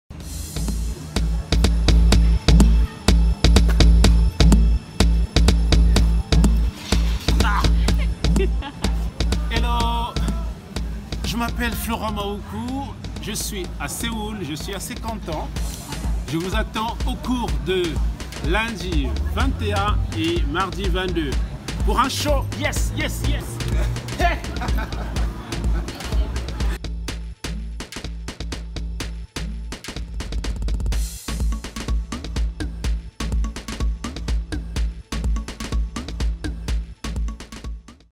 쿨레칸 스쿨 | 아프리카 안무가들 | 콩고 안무가 플로랑 마우쿠(Florent Mahoukou) 댄스워크숍
플로랑 마우쿠(Florent Mahoukou).
그는 스스로를 이렇게 정의한다.
서구 사회가 아프리카 예술가, 특히 댄서들에게 투영한 이미지를 깨부수는 무용가로서.

• LET IT EXPRESS 말하도록 놔둬 •
Florent Mahoukou Dance Workshop
On SEOUL, 2018

⚡️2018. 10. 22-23(월-화) 저녁 7:30-10:30
⚡️장소 : 에스꼴라 알레그리아(월), 플랫폼 A(화)
⚡️콩고 전통춤과 현대무용! 움직임 수업 2시간, 안무가와의 대화 1시간으로 이뤄질 예정입니다.

💥2010년 아프리카대륙 현대무용축제 Danse, L’afrique Danse 에서 작품 1위를 수상한 세계적인 안무가, 플로랑과의 춤을 놓치지 마세요! 바로 여기 서울에서! 😎